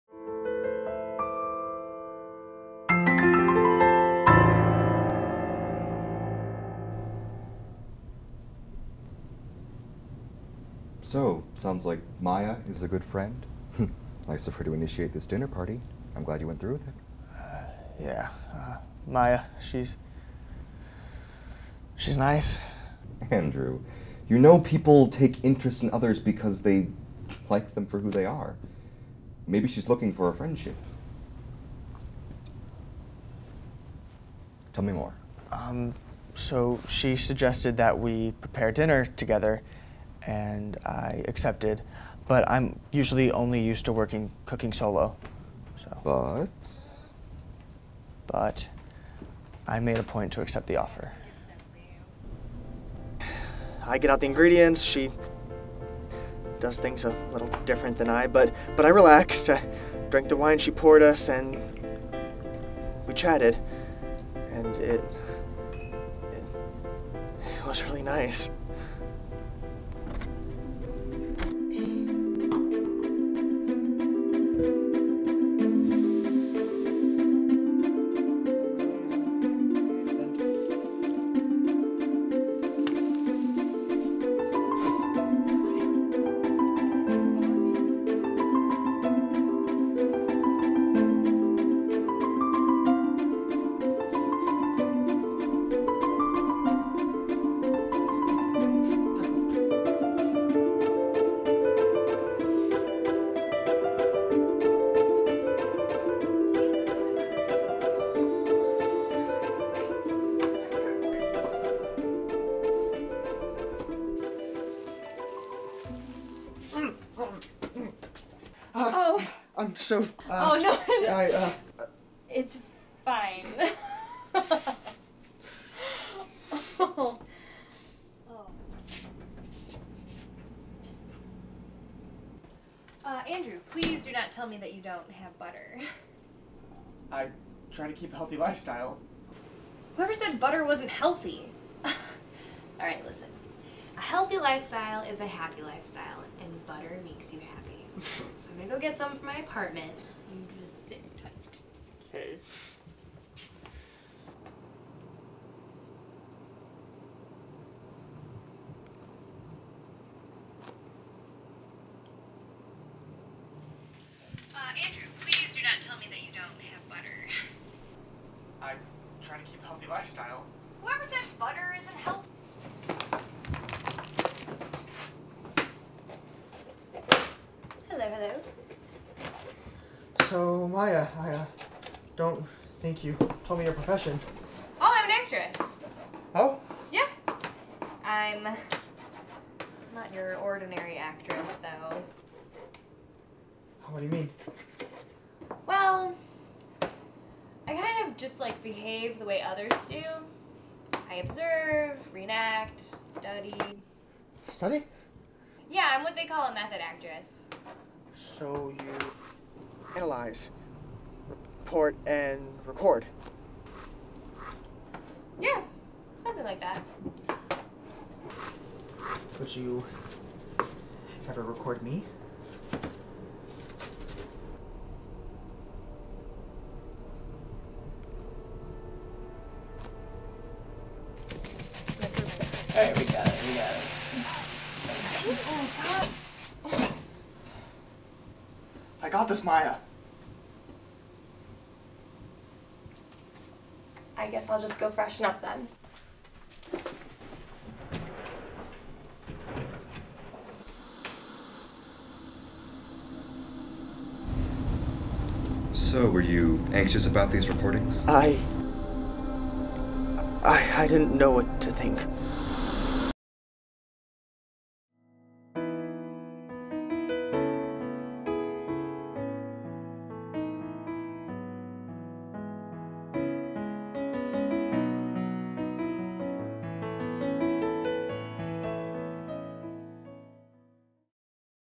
Mental Property Episode 2 "Boiling"
Written and Directed: Mack Neaton

Mental Property is a seven part web series surrounding a dinner party of five new floor mates getting to know each other hosted by a man with severe paranoia who is trying to get over it in therapy -- while discovering that each one of his new floor mates has their own similar tick.

In this episode, Andrew and Maia get to know each other a little more as they prepare good together for the dinner party.

Web Series Created for Television Department at Columbia College Chicago

"Be kind, for everyone you meet is fighting a harder battle." - Plato